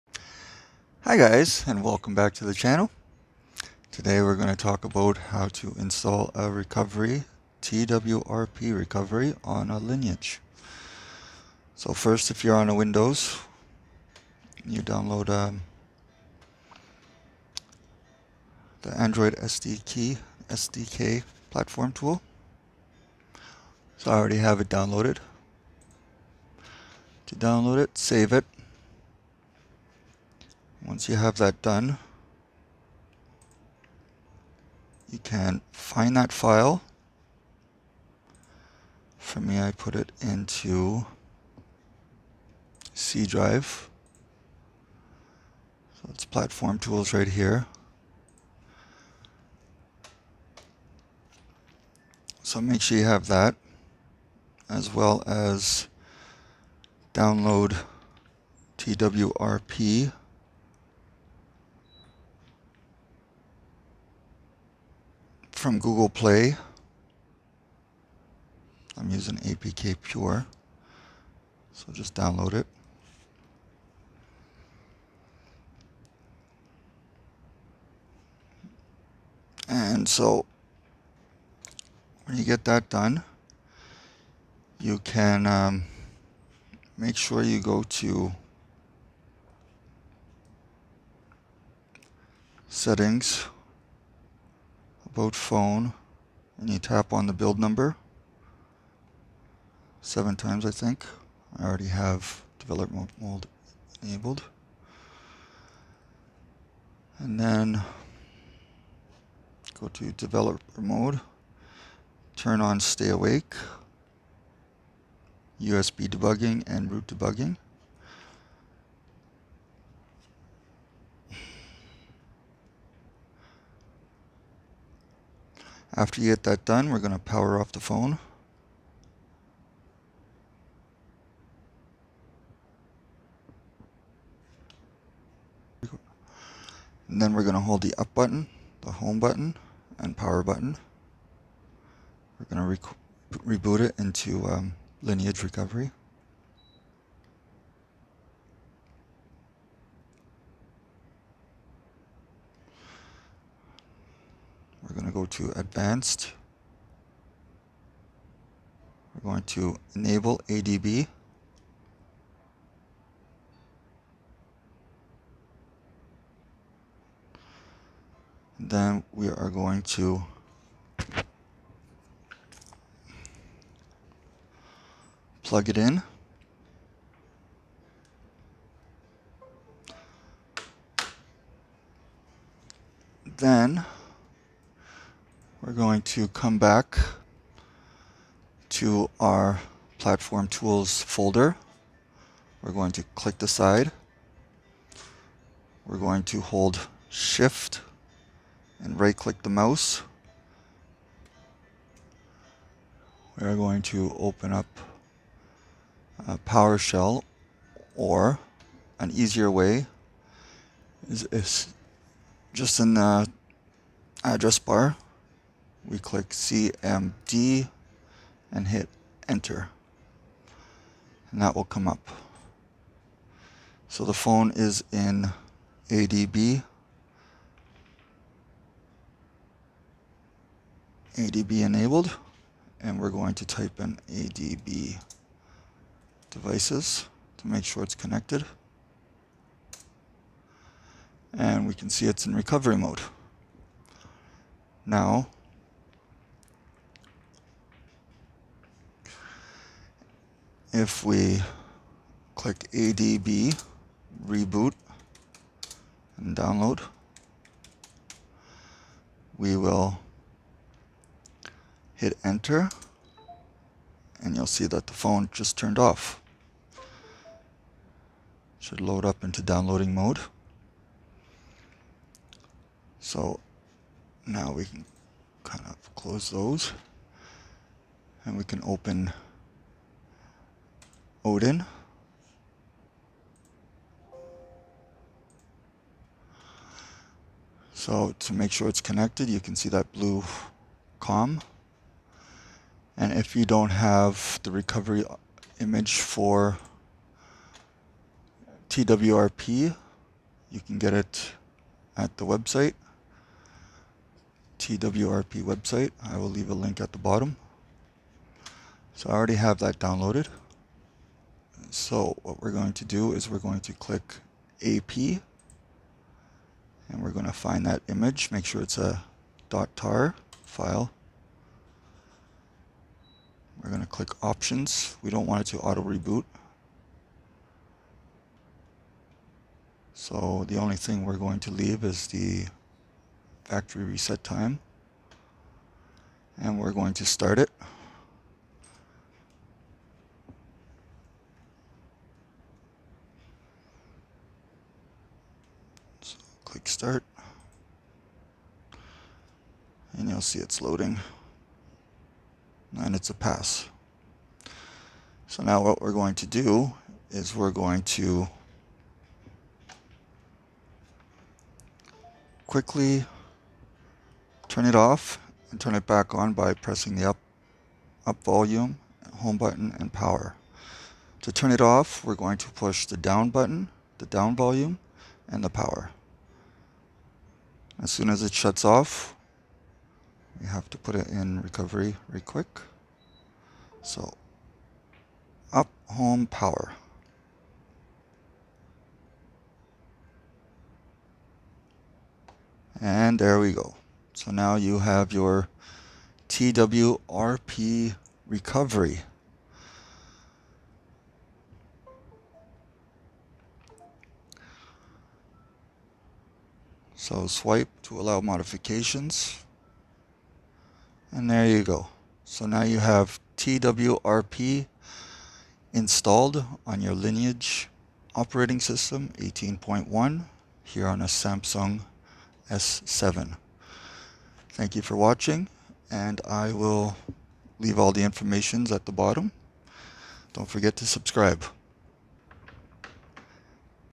Install TWRP Recovery on LineageOS 18.1 using Odin Tool. In this video we are using a Samsung S7.
1. (does not matter where the file is put).
2.
3. (I'm using Samsung S7 and the img.TAR file is
4.

It will NOT stay a Recovery for Lineage 18.1. Watch the next 2 videos (Root LineageOS 18.1 using Magisk Manager and TWRP Recovery & How to KEEP the TWRP Recovery on LineageOS 18.1 using Magisk) to learn how to have it your main recovery.